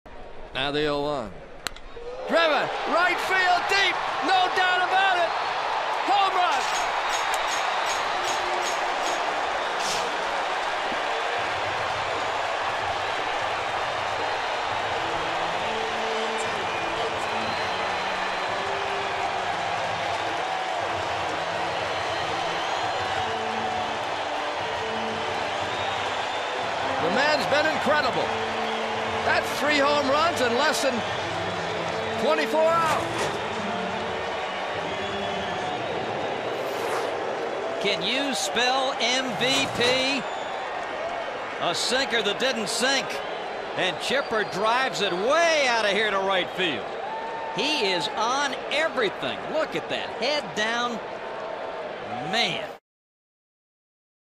NYM@ATL: Chipper takes Hershiser deep for 44th homer of 1999
9/22/99: Chipper Jones takes Orel Hershiser deep to right field for his 44th home run of the season